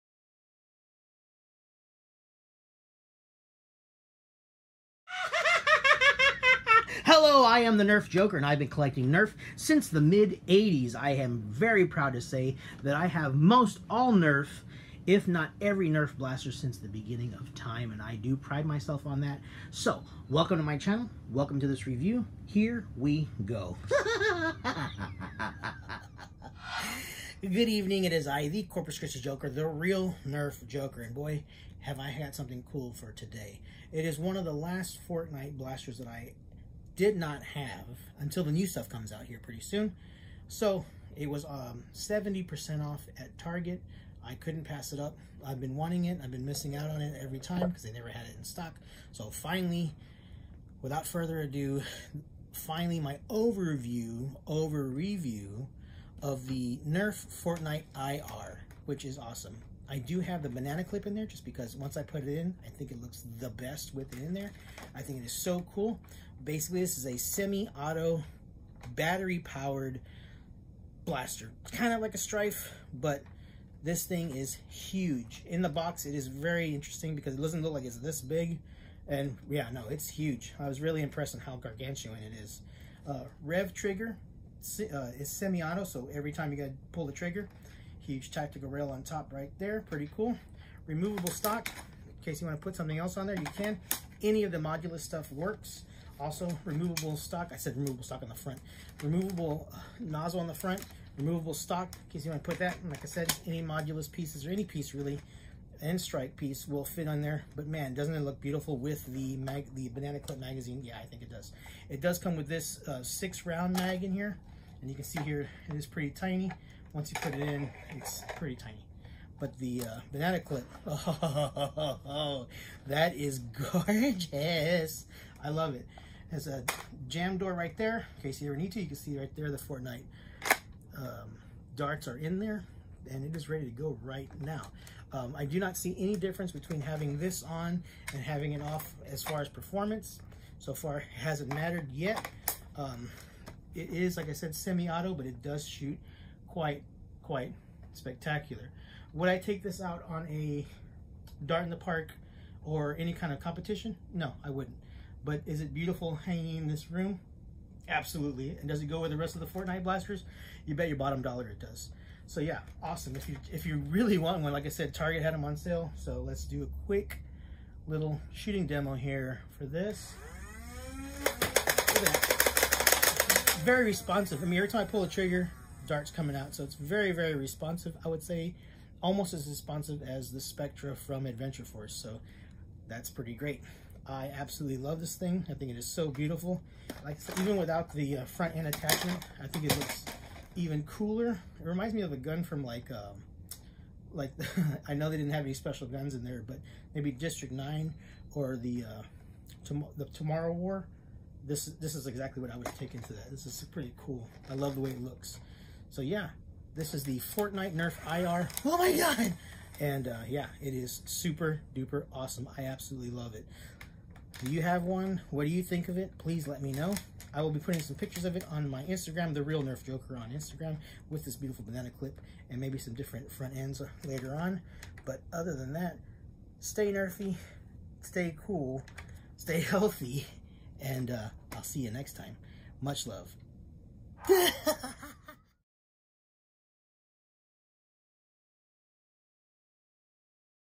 OverReview Nerf Fortnite IR
Here is the overreview for the new Nerf Fortnite IR, I def love this look and performance, let me know what u think! Don’t forget to check out my Facebook CorpusChristiJoker and my Instagram therealnerfjoker